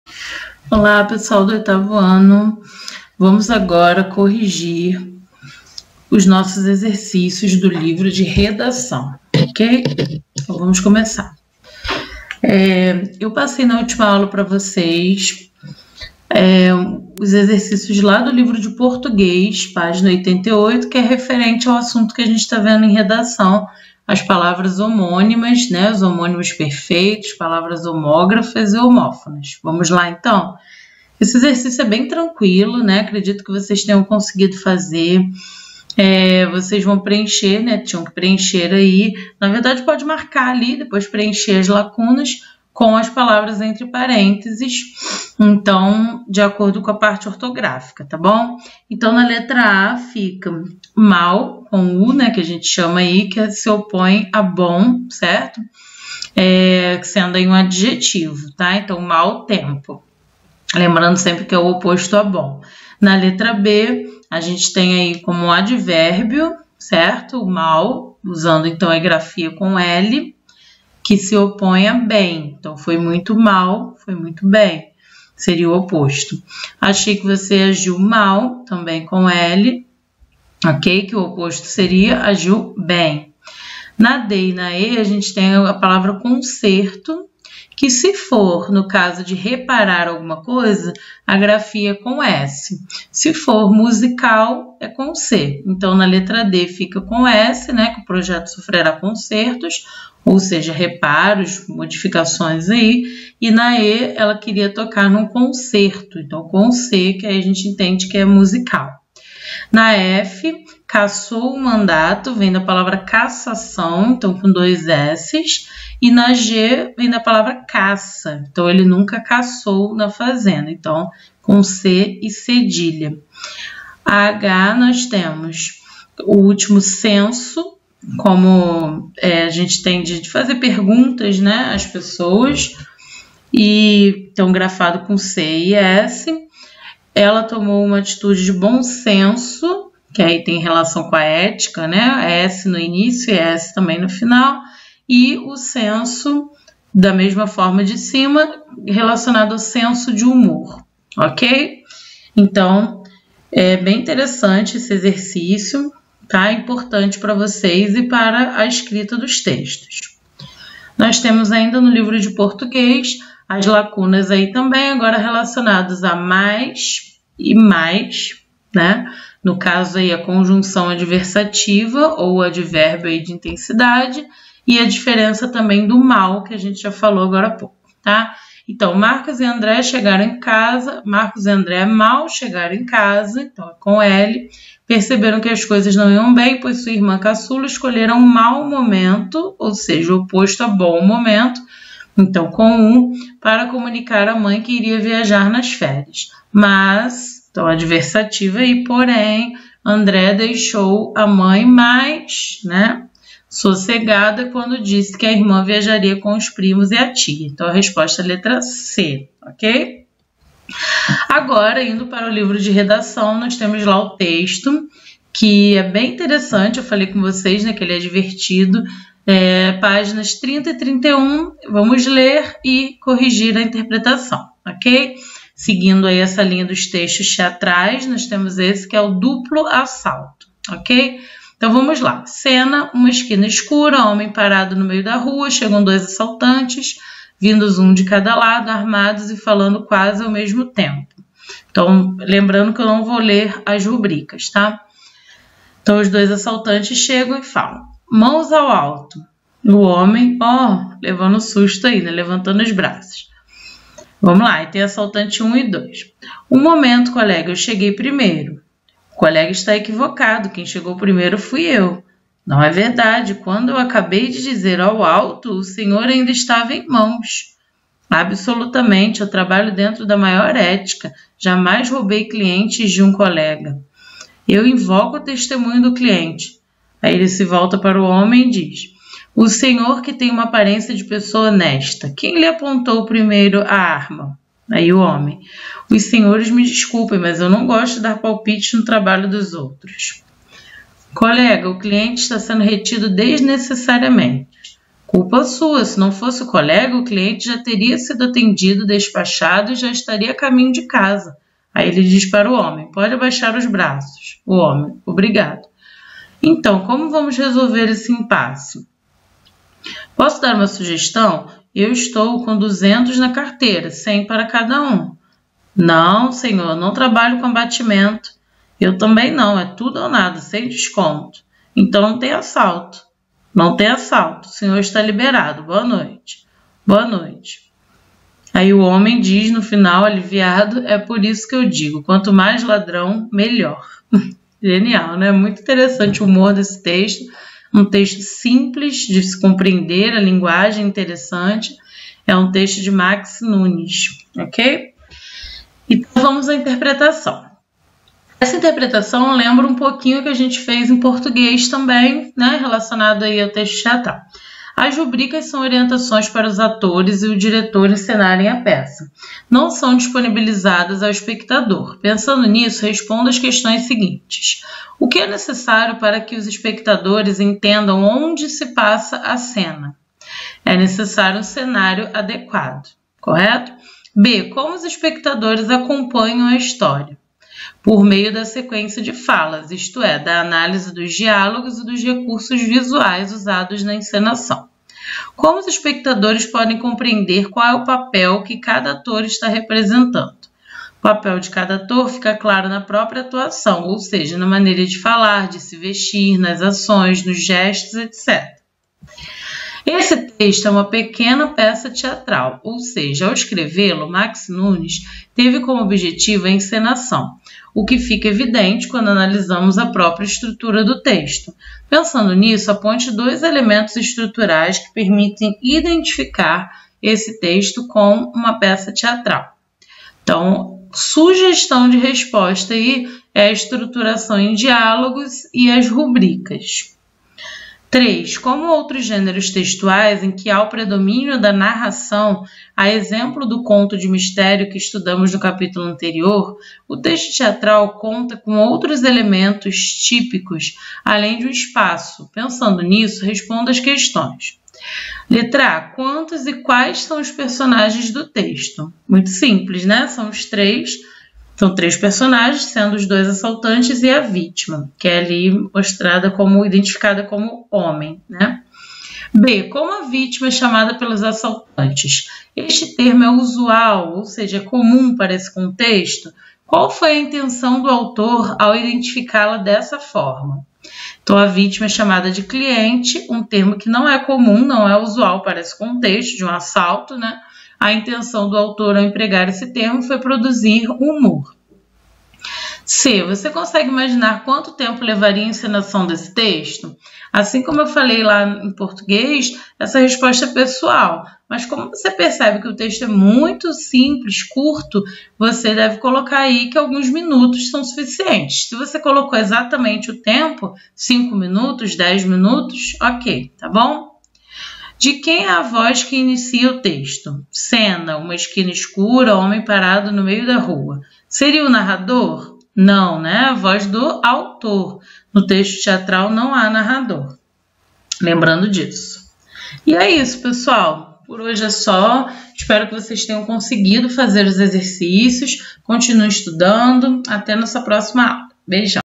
IAM - 8º ANO - REDAÇÃO (CORREÇÃO DAS ATIVIDADES DO LIVRO PÁGS. 30 E 31) 01/5
Correção das atividades do livro - 01/5
Professora: Débora Menezes